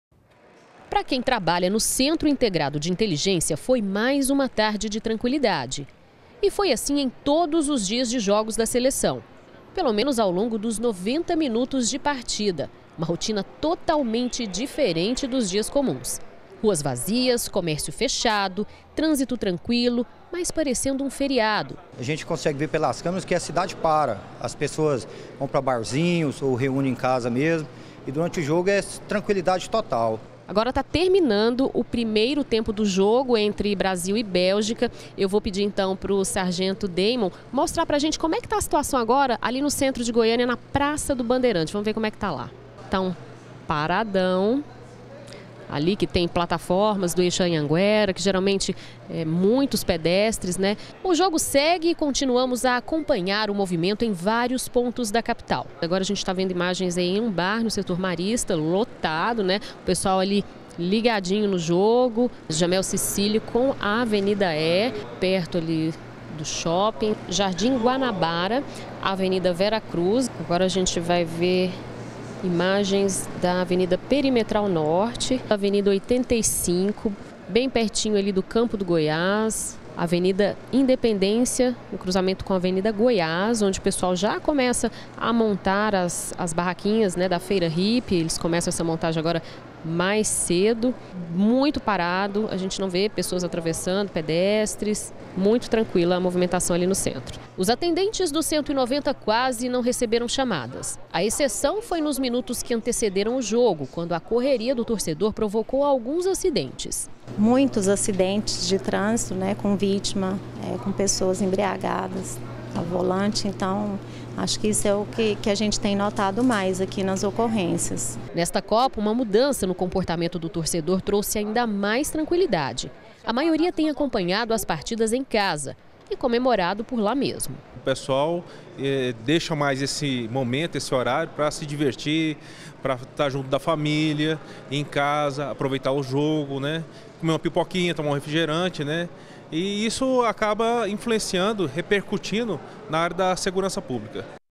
GR - Sem ocorrências: ruas vazias durante o jogo - 06-07-2018
Hoje durante o jogo as ruas de goiânia ficaram completamente desertas. Todo mundo parou pra sofrer com a seleção. Nem ocorrência policial teve, só um ou outro acidente provocado por bêbados, isso antes do jogo porque durante, nem isso.